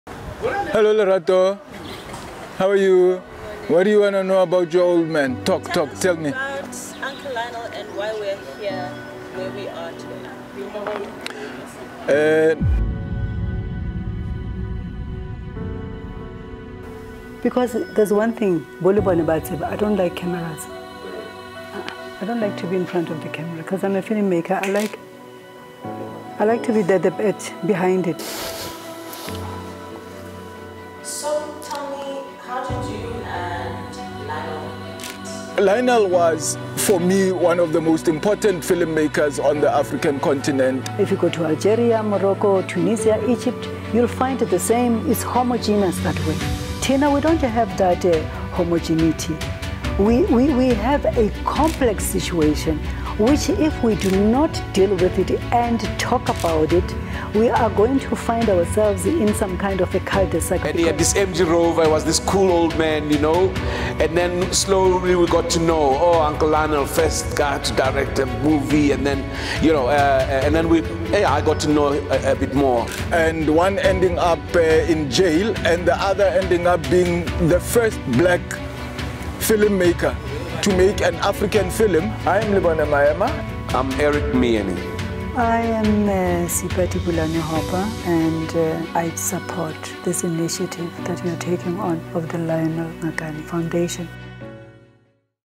MyLionel - The Story of Lionel Ngakane
July is a special month for the foundation. The 17th is Lionel Ngakane’s birthday 🎉, and what better way to celebrate him than to get to know him better?

We spoke to Eric Miyeni, Lebone Maema and Seipati Bulane-Hopa (all great minds in the South African film industry) about Lionel Ngakane and we’re excited to share our conversations with you. The interviews will premier on our YouTube channel on July 17th.